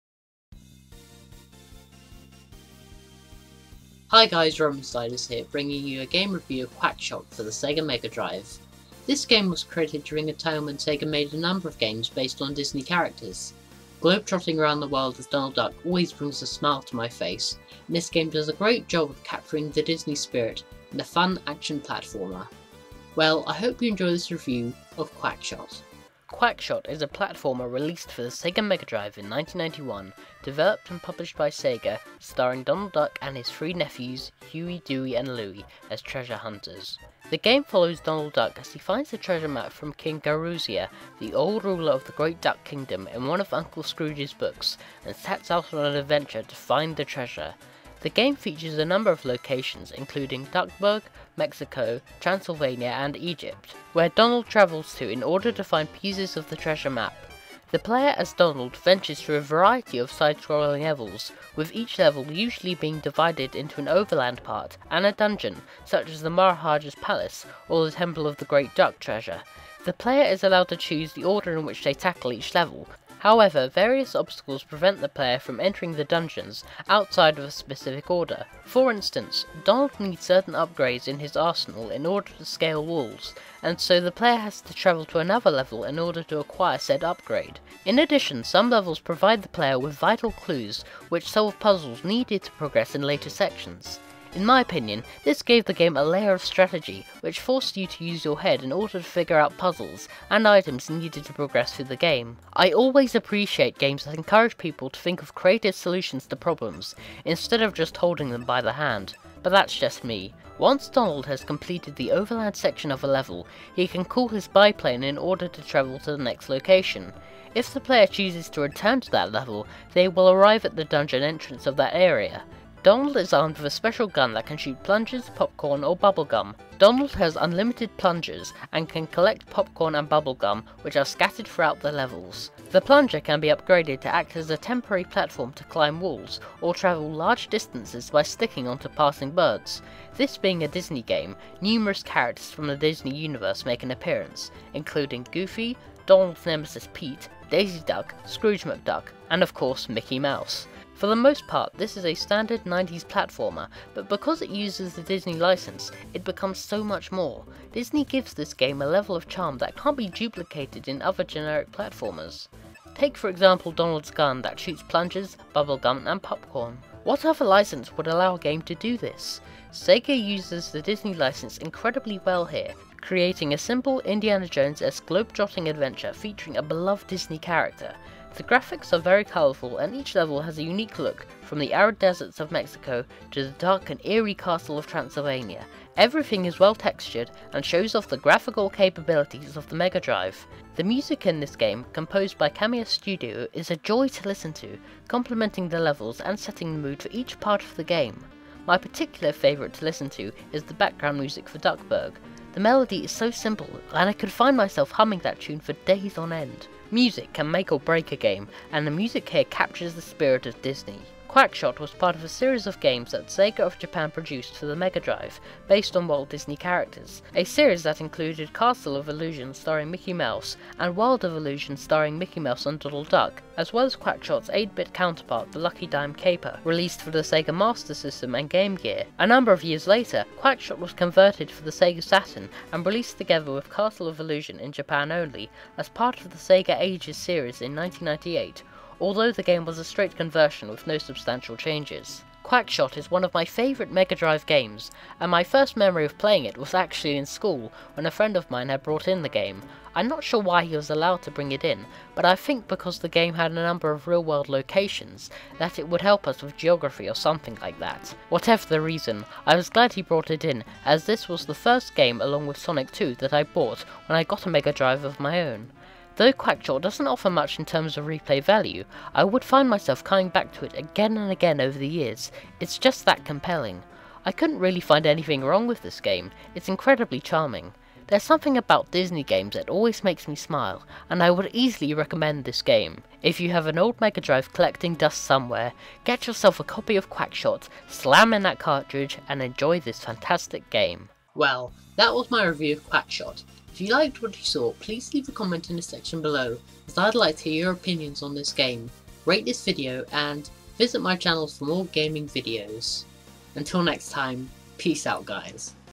The Reviewz - QuackShot Review
A review of QuackShot for the Sega Mega Drive, released in 1991. New episodes of Gaming Rewind/The Reviewz every Wednesday, Friday and Sunday.